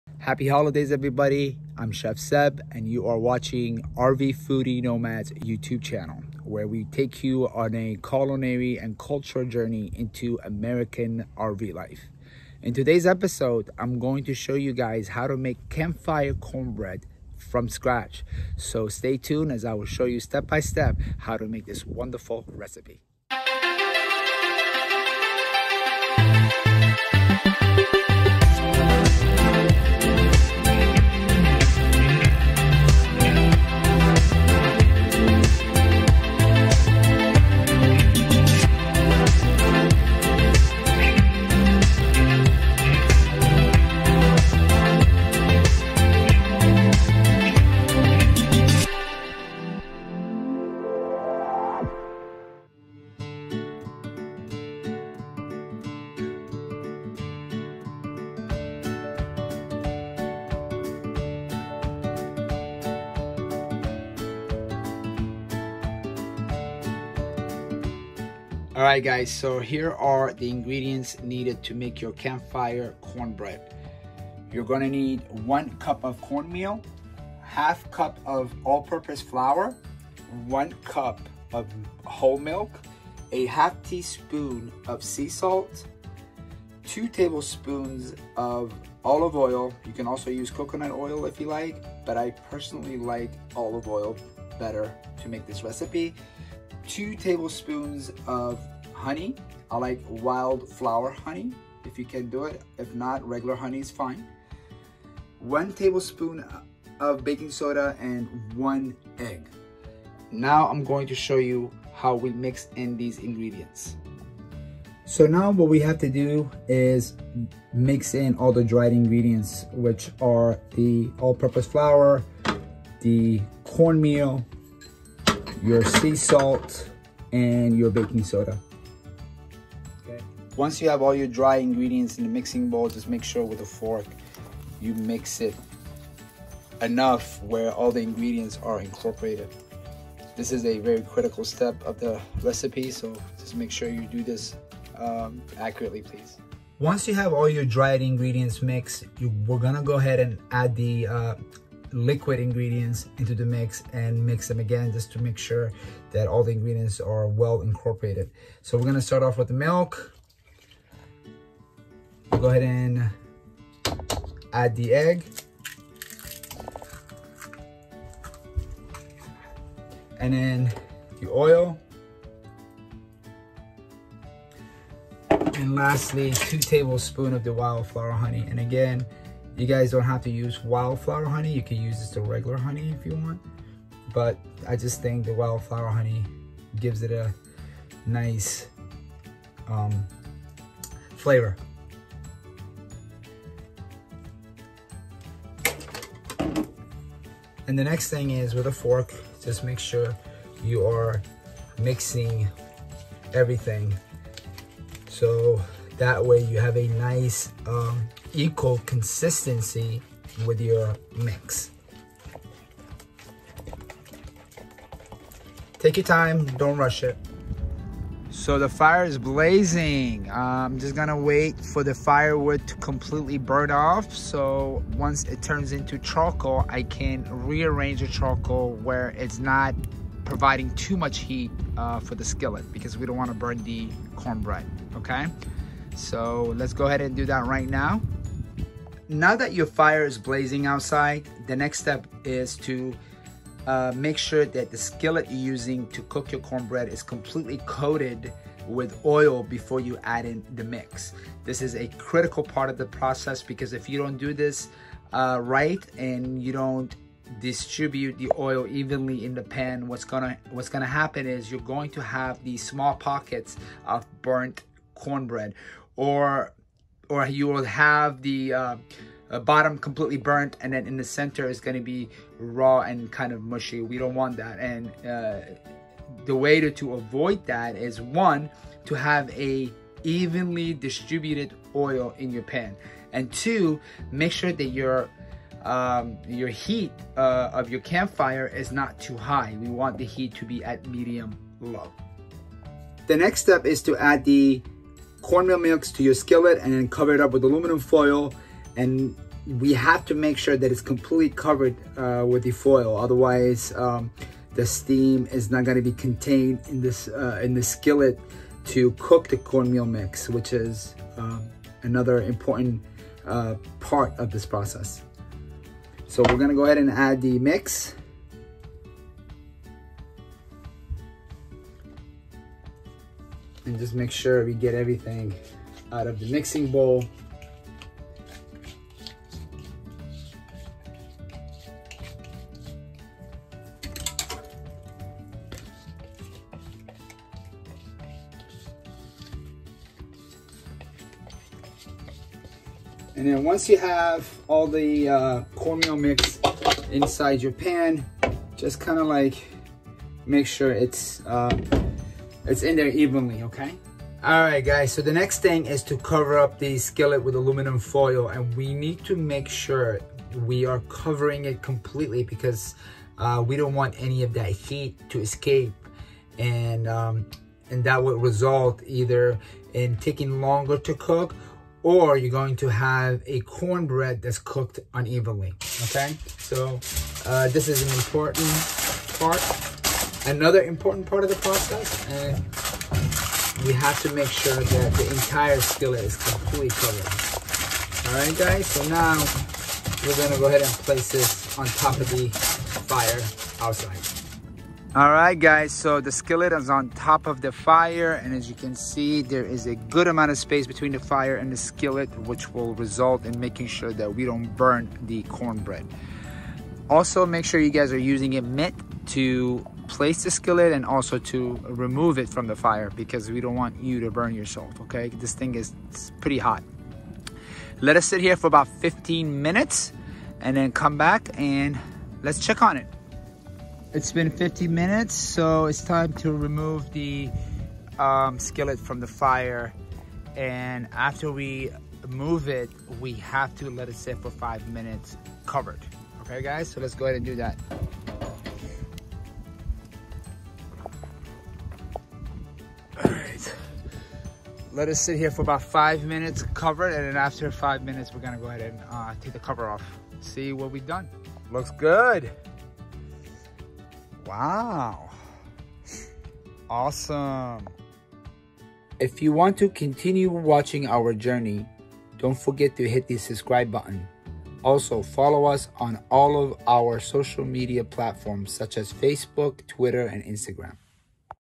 Fall Camping - Campfire Cornbread Recipe
In this week’s episode, Chef Seb demonstrates how you can make his tasty cornbread using a cast iron skillet at your campfire. Watch this easy step by step video to learn how to make the perfect bread to go with the next cup of chili this fall and winter!